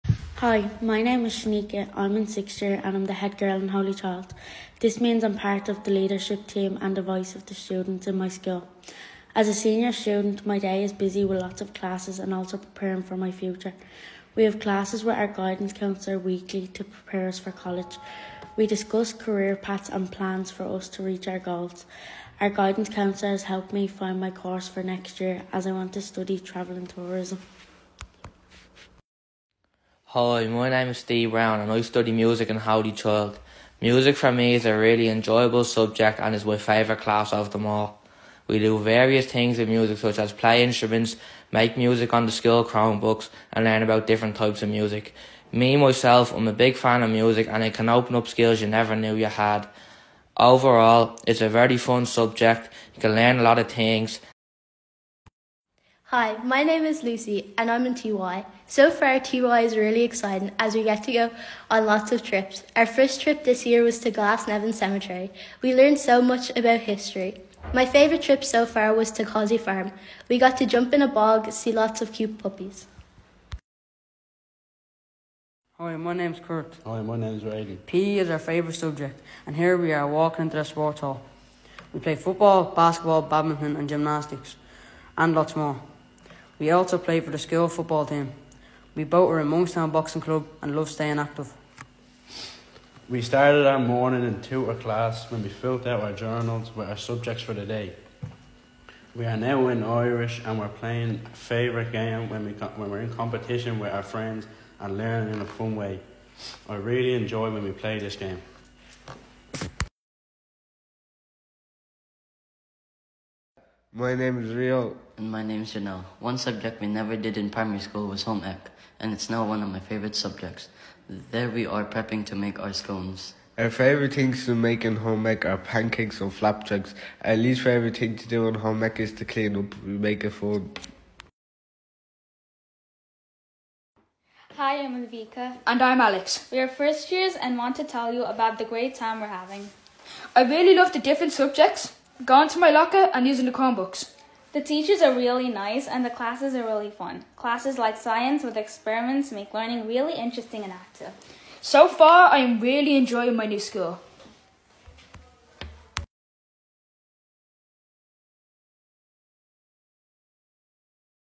A day in the life of a Holy Child CS student
A video following some HCCS students on their daily life in secondary school! We meet fantastic students from juniors to seniors and get an insight into our school!